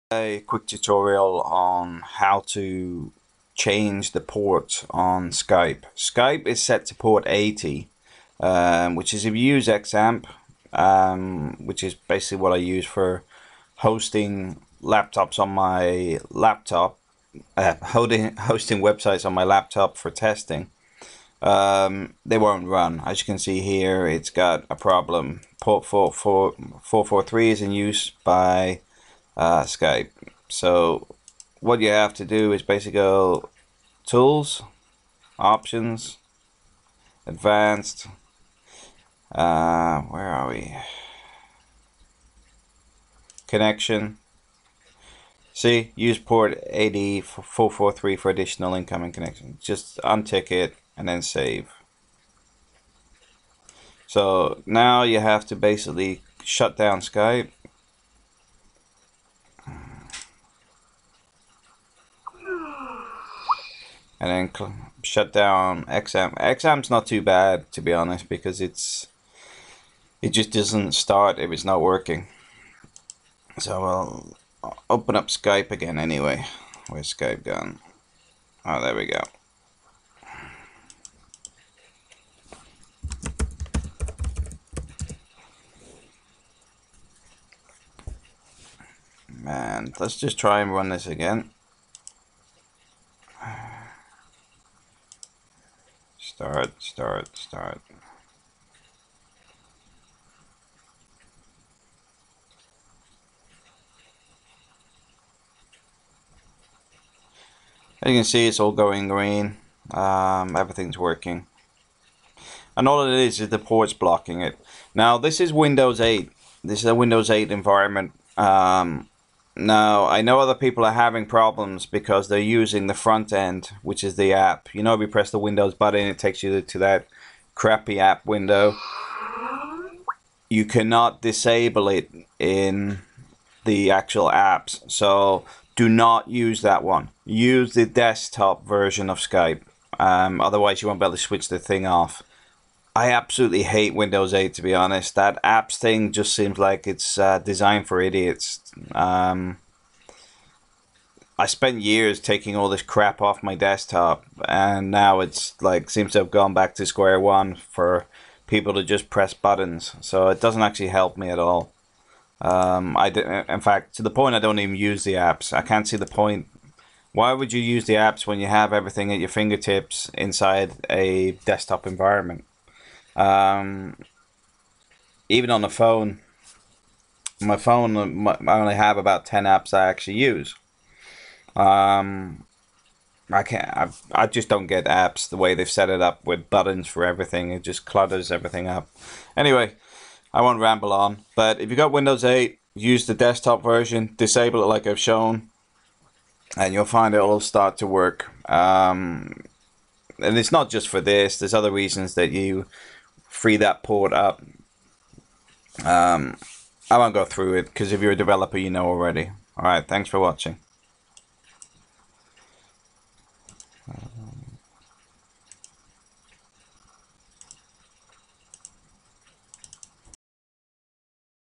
How to change ports on Skype for Windows 8
If your a windows 8 user and in development etc. You may be finding you can't change the ports or wondering how you change ports for Skype. The reality is that Skype hogs ports 443 and 80 and as such some other software can't work as they need the same ports. This quick tutorial shows you how to change your ports. But also if your using the "App" side of Windows 8 you need to change to the desktop version of Skype. They haven't allowed the access to ports on the App.